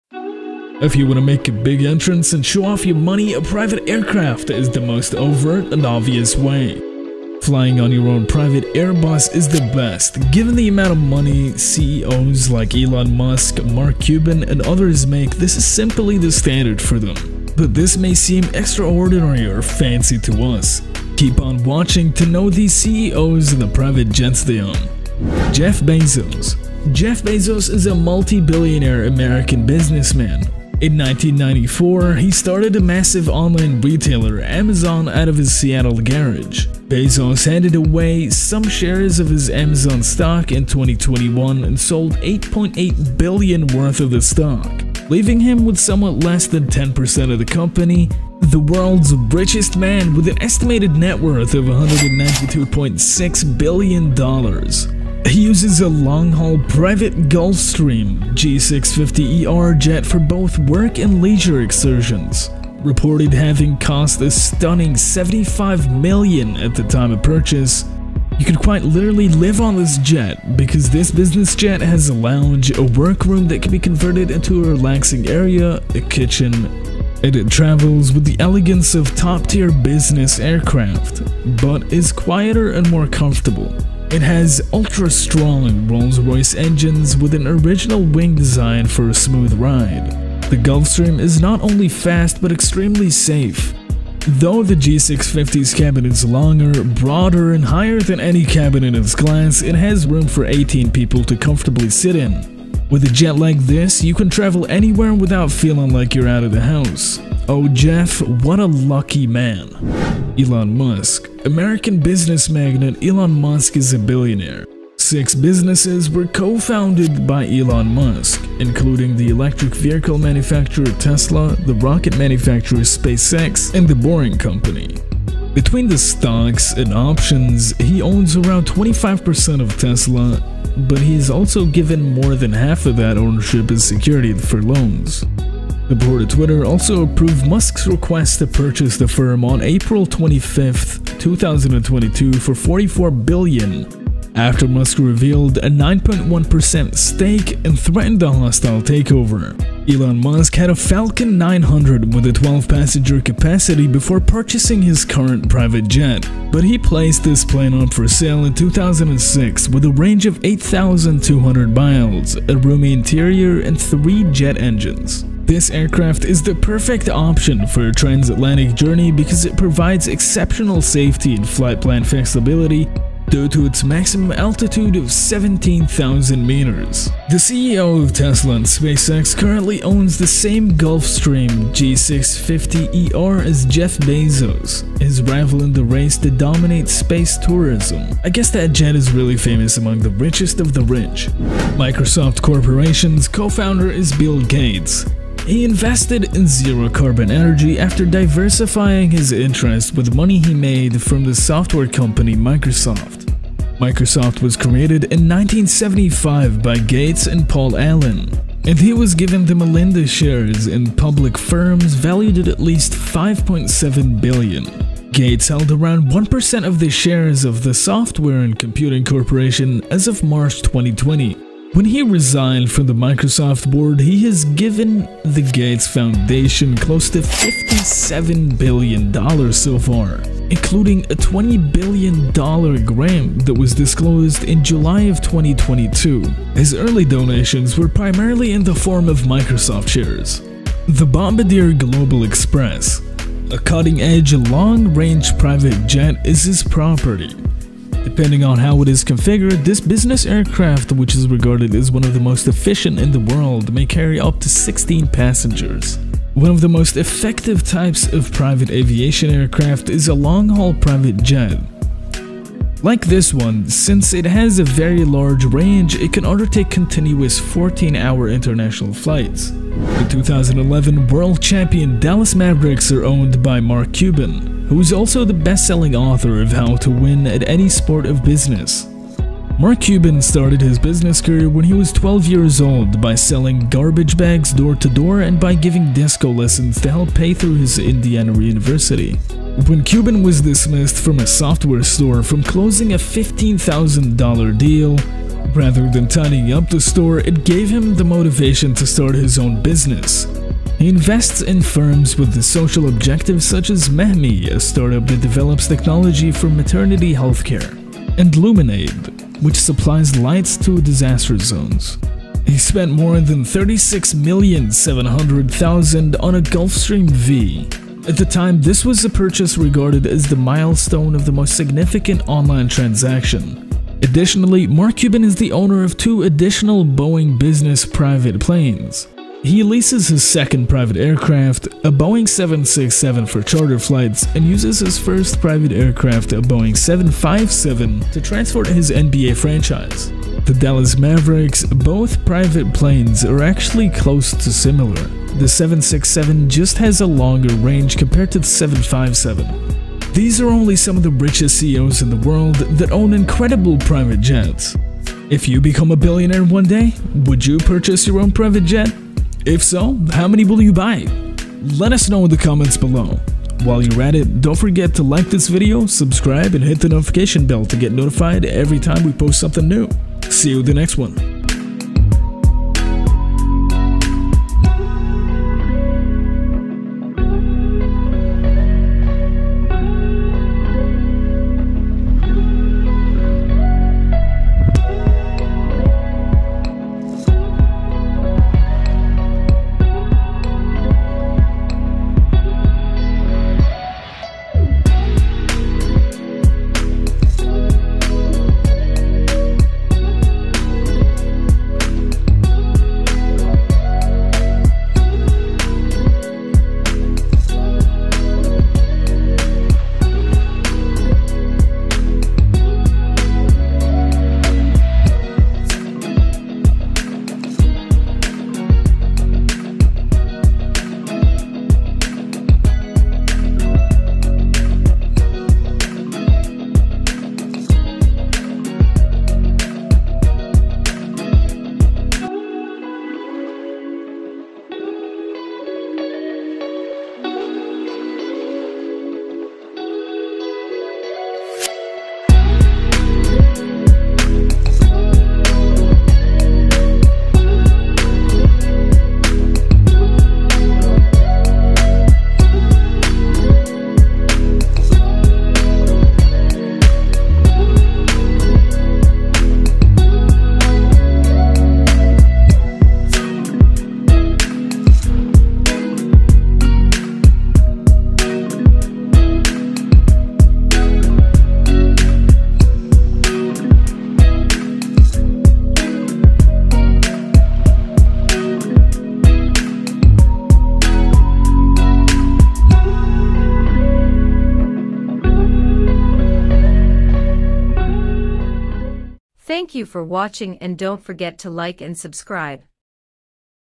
Trapped Inside the Secret World of Billionaire Air Travel!
"Have you ever wondered how the world's richest CEOs travel around the globe? In this video, we take a look at the luxurious private jets that some of the wealthiest business leaders own and use for their personal and professional trips. From the sleek and modern designs of tech moguls like Jeff Bezos and Mark Zuckerberg, to the classic and elegant styles of fashion icons like Ralph Lauren and Giorgio Armani, these private jets are not only symbols of wealth and success, but also of comfort and convenience. Join us on a journey through the skies and discover the fascinating world of private aviation for the elite few.